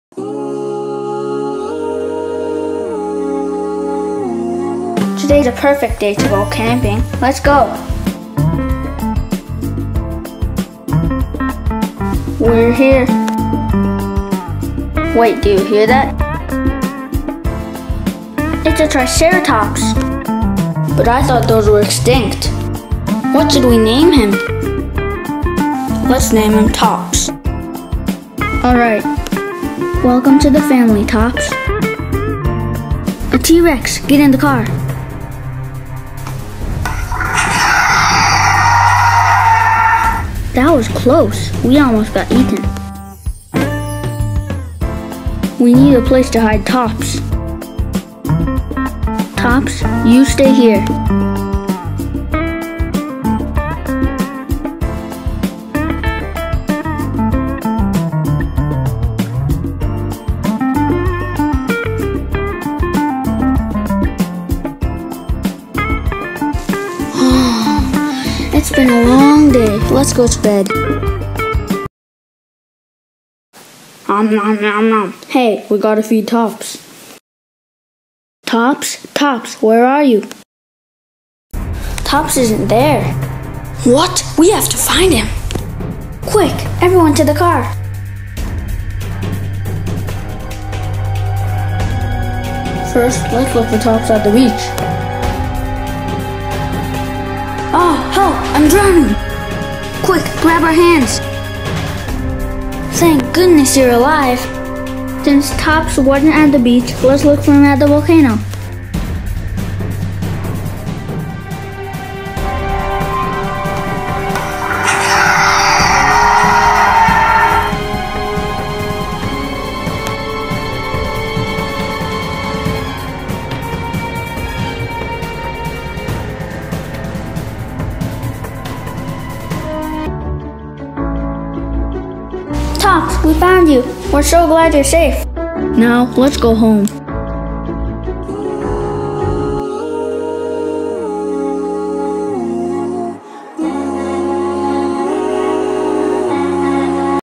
Lego stop motion movie: Tops-A Triceratops story
In this epic stop motion movie, a family struggles to keep a Triceratops safe from the dangerous things-like the T-rex...